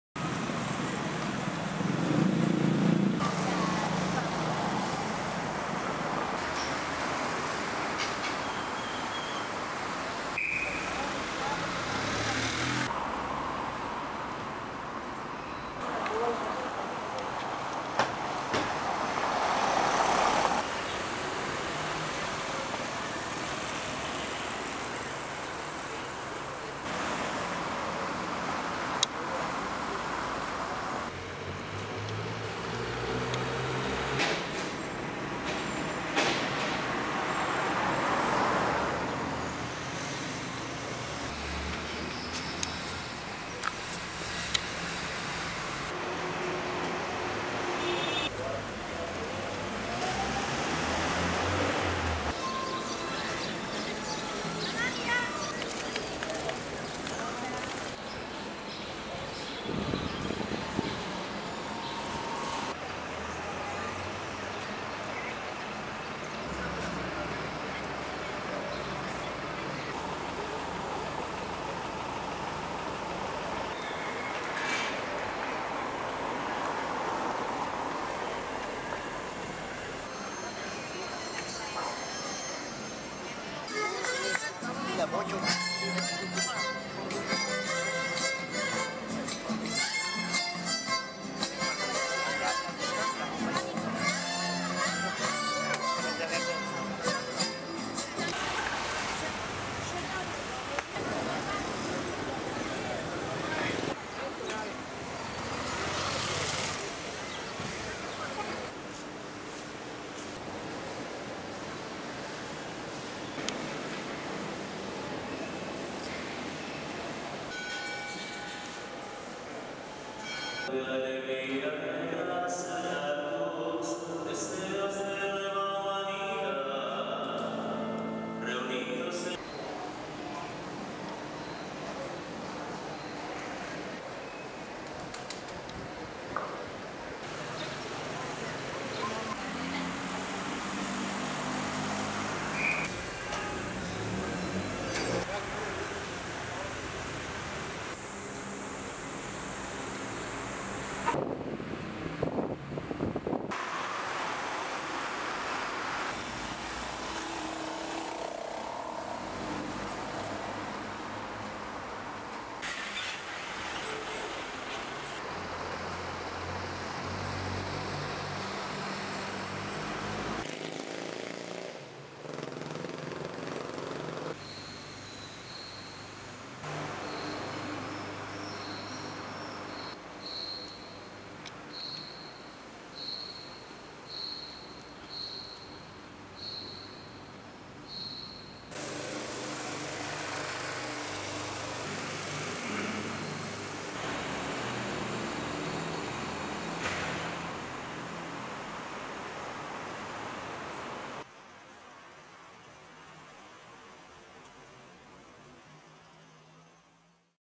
SALTA: Centro histórico, catedral e Iglesia de San Francisco
Recorrido por el centro histórico de la ciudad de SALTA